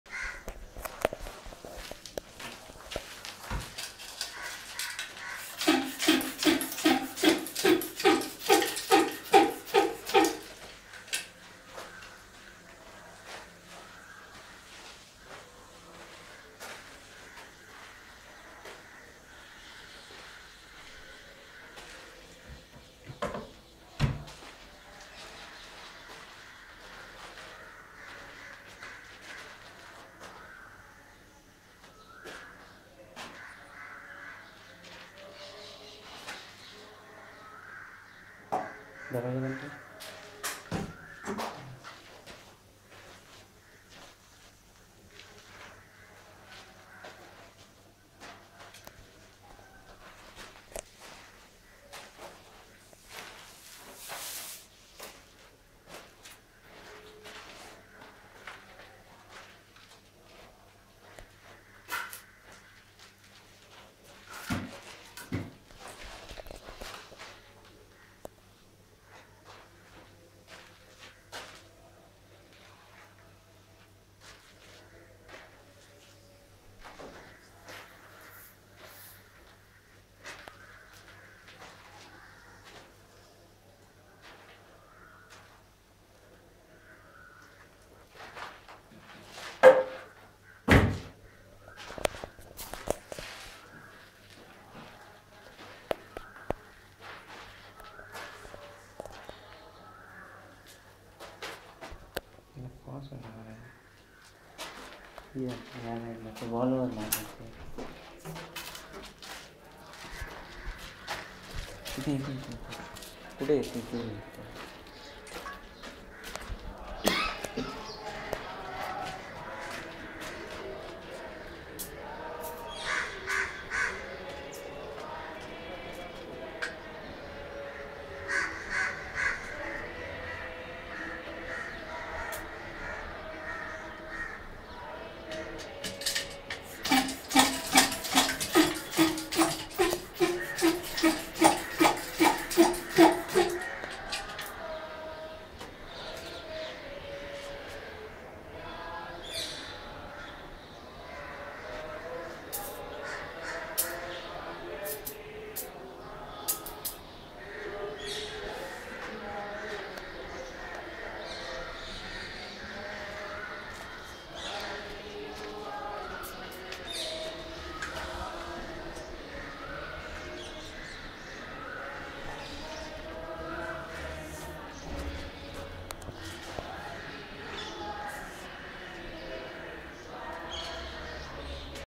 Pre Construction Termite Control treatment
Termite Control - Anti Termite treatment by treating the bare flooring. In bare flooring treatment the chemical get absorbed quickly and uniform layer is formed which creates a good chemical barrier.